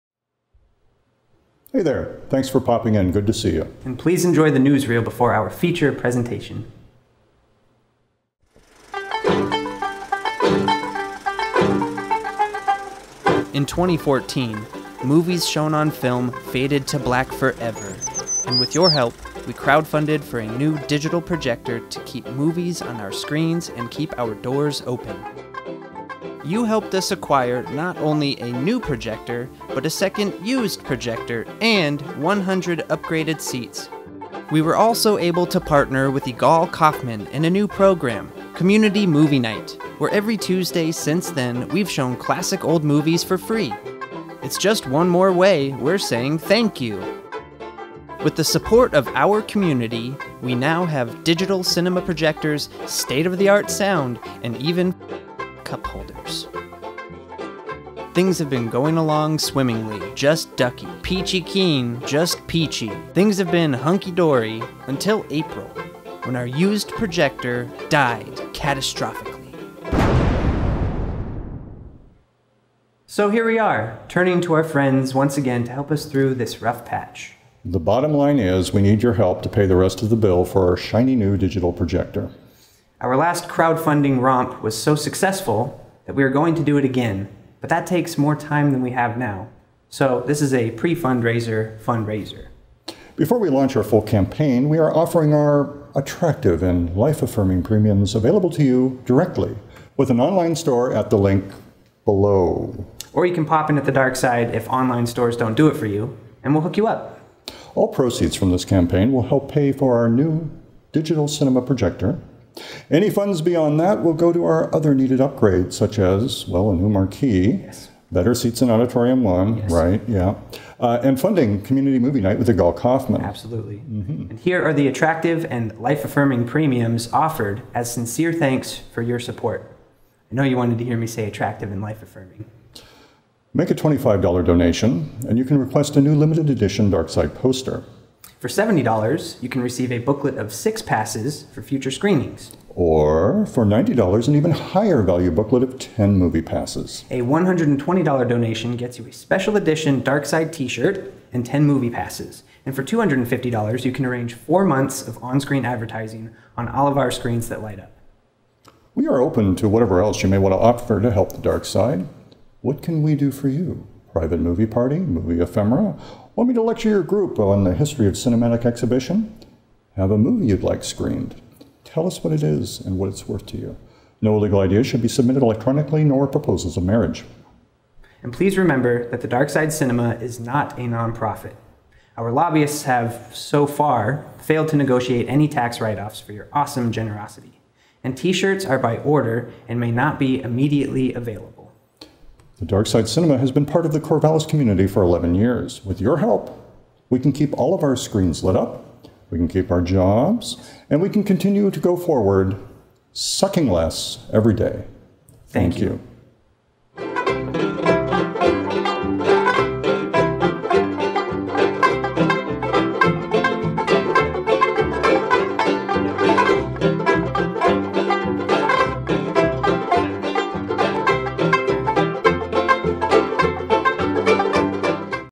Darkside Pre-Fundraiser Fundraiser
We're passing the hat again, kids. We need to pay for the new projector. We need your help. Click the link below to do your part.

Though we can fix most film projector problems with baling wire and sarcasm, we have had to rely on powers other than our own to keep digital light on the screens. So when our used digital cinema projector (DCP) died, then died again, we ran out of bailing wire. We had to get a new projector. Brand spankin’ new. We need your help.